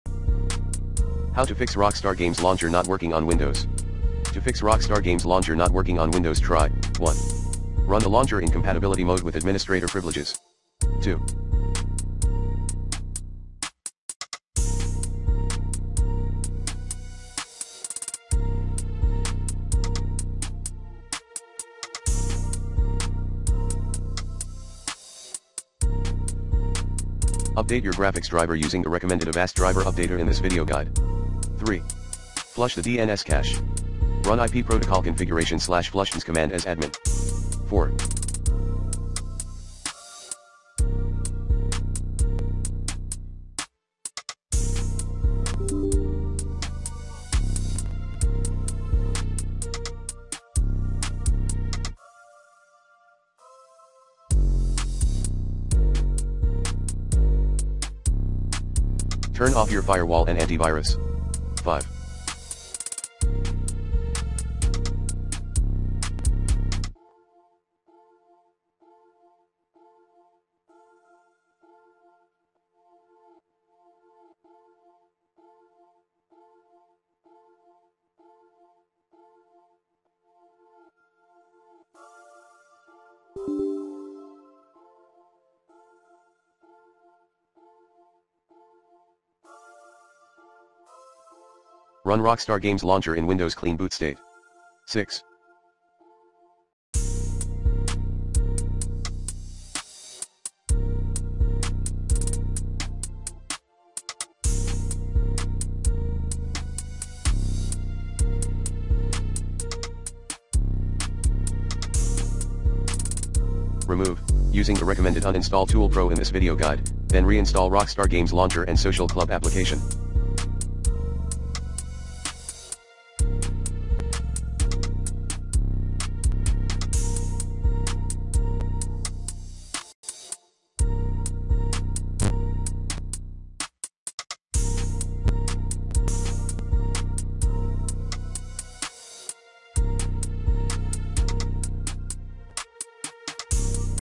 Fix Rockstar Games Launcher not working on Windows 11/10 (2023 Updated)
Here's how to Fix Rockstar Games Launcher not working on Windows 11/10. Run Avast Driver Updater@ to keep your device drivers up-to-date, easily & effectively. To effectively remove unwanted/problematic programs, run this best uninstall tool@

Here are some troubleshooting steps to get the Rockstar Games Launcher working again on Windows:

- Check for Windows updates and install any available updates. Restart after installing.

- Temporarily disable antivirus software. Some antivirus can block the Rockstar Launcher. Re-enable after testing.

- Make sure you have the latest .NET Framework installed. The Rockstar Launcher requires .NET Framework 4.5 or higher.

- Open Services and check if Rockstar Games Launcher Service is running. If not, right click and start the service. Set to Automatic start.

- Navigate to C:\Program Files\Rockstar Games\Launcher and run LauncherPatcher.exe. This may update the launcher files.

- Open Task Manager while trying to launch and check for any error messages. This could indicate why it's not opening.

- Uninstall the Rockstar Launcher, restart your PC, then download and reinstall the latest version of the launcher.

- Delete any Rockstar Games folders in Documents and AppData/Local to clear out corrupted data.

- As last resort, perform clean reinstall of Windows to eliminate any OS related issues.

- Check Rockstar Support site for any known launcher issues. Contact their support if problem persists.

Hopefully one of those steps will resolve the issue with the Rockstar Games Launcher on your Windows system.

i. Here is a list of some common issues that can occur with the Rockstar Games Launcher and potential fixes:

- Launcher won't open - Reinstall launcher, update Windows/.NET Framework, run as admin, allow in firewall

- Games won't launch - Reinstall game, verify files, update graphics drivers, disable overlays

- Downloads are slow - Pause other downloads, disable VPN, open ports, try wired connection

- Can't login/connect - Check internet connection, reboot router, allow launcher in antivirus

- Errors on launch - Update Windows, run launcher as admin, compatibility troubleshooting

- Game crashes - Update graphics drivers, lower settings, disable overlays/background apps

- Social Club issues - Clear cache/data folder, reinstall Social Club, disconnect VPN

- Installation errors - Disable antivirus, run installer as admin, install prerequisites

- Can't locate game files - Add exception to antivirus, check permissions on game folders

- Corrupted files - Verify game files through launcher, re-download affected files

- High CPU/RAM usage - Close other apps, upgrade PC hardware, adjust game settings

Refer to Rockstar Support site for help on specific error codes. Regularly installing Windows and driver updates can prevent many issues.